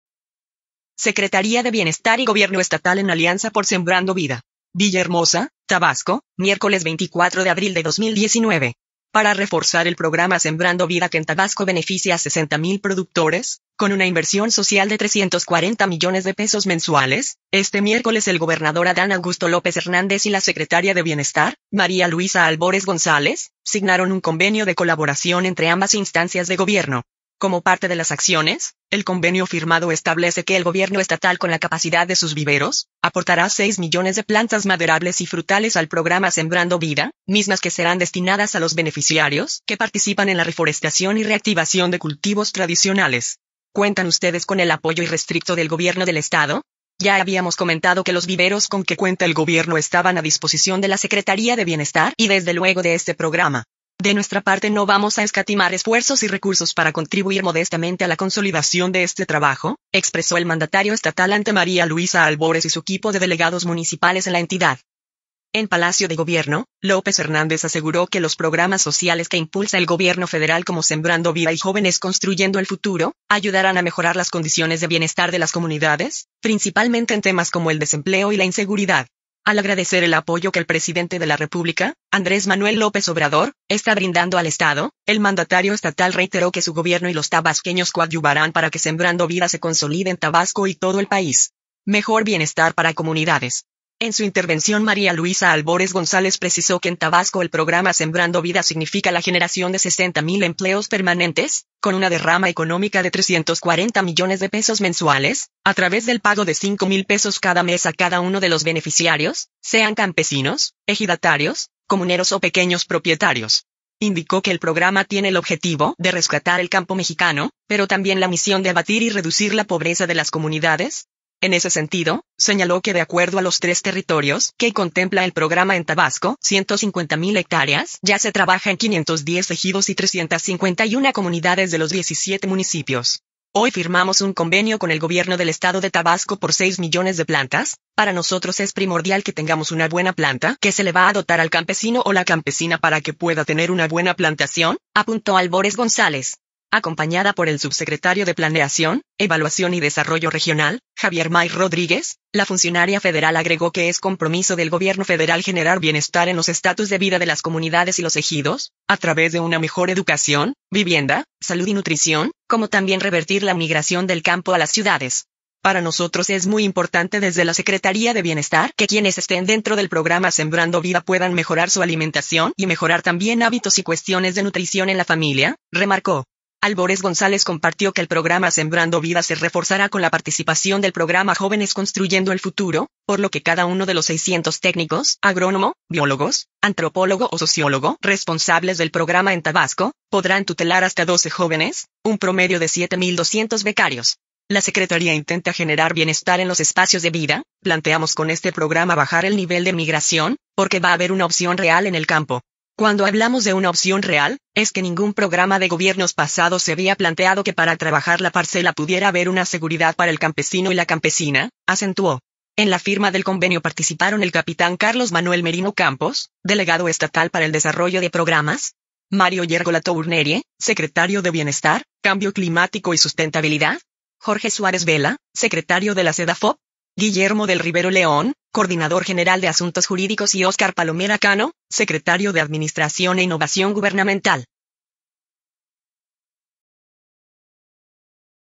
Secretaría de Bienestar y Gobierno Estatal en alianza por Sembrando Vida
Villahermosa, Tabasco, miércoles 24 de abril de 2019.

Para reforzar el programa Sembrando Vida que en Tabasco beneficia a 60 mil productores, con una inversión social de 340 millones de pesos mensuales, este miércoles el gobernador Adán Augusto López Hernández y la secretaria de Bienestar, María Luisa Albores González, signaron un convenio de colaboración entre ambas instancias de gobierno.

Como parte de las acciones, el convenio firmado establece que el Gobierno Estatal con la capacidad de sus viveros, aportará seis millones de plantas maderables y frutales al programa Sembrando Vida, mismas que serán destinadas a los beneficiarios que participan en la reforestación y reactivación de cultivos tradicionales.

“Cuentan ustedes con el apoyo irrestricto del Gobierno del Estado; ya habíamos comentado que los viveros con que cuenta el Gobierno estaban a disposición de la Secretaría de Bienestar y desde luego de este programa. De nuestra parte no vamos a escatimar esfuerzos y recursos para contribuir modestamente a la consolidación de este trabajo”, expresó el mandatario estatal ante María Luisa Albores y su equipo de delegados municipales en la entidad.

En Palacio de Gobierno, López Hernández aseguró que los programas sociales que impulsa el Gobierno Federal como Sembrando Vida y Jóvenes Construyendo el Futuro, ayudarán a mejorar las condiciones de bienestar de las comunidades, principalmente en temas como el desempleo y la inseguridad.

Al agradecer el apoyo que el presidente de la República, Andrés Manuel López Obrador, está brindando al Estado, el mandatario estatal reiteró que su Gobierno y los tabasqueños coadyuvarán para que Sembrando Vida se consolide en Tabasco y todo el país.

Mejor bienestar para comunidades.

En su intervención María Luisa Albores González precisó que en Tabasco el programa Sembrando Vida significa la generación de 60 mil empleos permanentes, con una derrama económica de 340 millones de pesos mensuales, a través del pago de cinco mil pesos cada mes a cada uno de los beneficiarios, sean campesinos, ejidatarios, comuneros o pequeños propietarios.

Indicó que el programa tiene el objetivo de rescatar el campo mexicano, pero también la misión de abatir y reducir la pobreza de las comunidades; en ese sentido, señaló que de acuerdo a los tres territorios que contempla el programa en Tabasco (150 mil hectáreas) ya se trabaja en 510 ejidos y 351 comunidades de los 17 municipios.

“Hoy firmamos un convenio con el Gobierno del Estado de Tabasco por seis millones de plantas, para nosotros es primordial que tengamos una buena planta que se le va a dotar al campesino o la campesina para que pueda tener una buena plantación”, apuntó Albores González.

Acompañada por el subsecretario de Planeación, Evaluación y Desarrollo Regional, Javier May Rodríguez, la funcionaria federal agregó que es compromiso del Gobierno Federal generar bienestar en los estatus de vida de las comunidades y los ejidos, a través de una mejor educación, vivienda, salud y nutrición, como también revertir la migración del campo a las ciudades.

“Para nosotros es muy importante desde la Secretaría de Bienestar que quienes estén dentro del programa Sembrando Vida puedan mejorar su alimentación y mejorar también hábitos y cuestiones de nutrición en la familia”, remarcó.

Albores González compartió que el programa Sembrando Vida se reforzará con la participación del programa Jóvenes Construyendo el Futuro, por lo que cada uno de los 600 técnicos (agrónomo, biólogos, antropólogo o sociólogo) responsables del programa en Tabasco, podrán tutelar hasta 12 jóvenes, un promedio de siete mil 200 becarios.

“La Secretaría intenta generar bienestar en los espacios de vida, planteamos con este programa bajar el nivel de migración, porque va haber una opción real en el campo. Cuando hablamos de una opción real, es que ningún programa de gobiernos pasados se había planteado que para trabajar la parcela pudiera haber una seguridad para el campesino y la campesina”, acentuó.

En la firma del convenio participaron el capitán Carlos Manuel Merino Campos, delegado estatal para el Desarrollo de Programas; Mario Llergo Latournerie, secretario de Bienestar, Cambio Climático y Sustentabilidad; Jorge Suárez Vela, secretario de la Sedafop; Guillermo del Rivero León, coordinador general de Asuntos Jurídicos y Óscar Palomera Cano, secretario de Administración e Innovación Gubernamental.